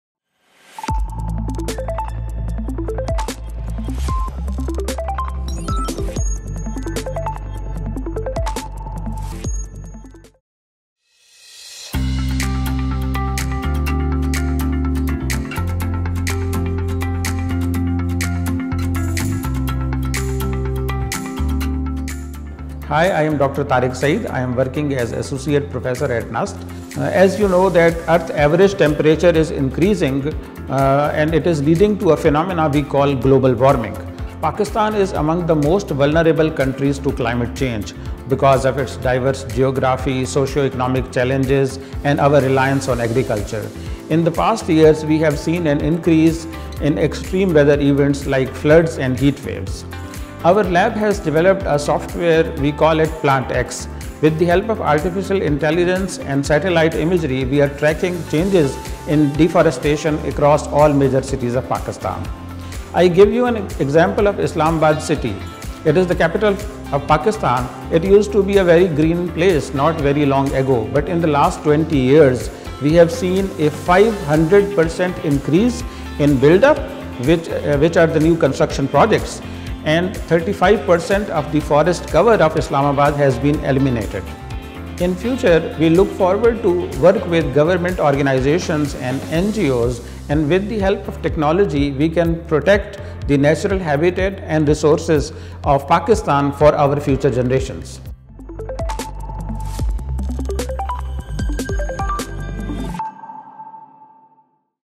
Episode 18 | Mapping Tree Plantation with Smart Tracking @Research_NUST | Innovatio Impulsum
Meet PlantX, an innovative web-based application developed to streamline and enhance tree plantation efforts. 🌍

From real-time monitoring and automated progress tracking to data-driven decision-making, PlantX centralizes all your plantation data into a single smart dashboard. With features like site classification, visual documentation, progress analytics, and collaborative tools, this platform empowers conservation stakeholders to work efficiently and transparently.

Whether you're planting a few trees or managing large-scale reforestation drives, PlantX ensures every tree is counted and cared for. Join the movement toward a sustainable future.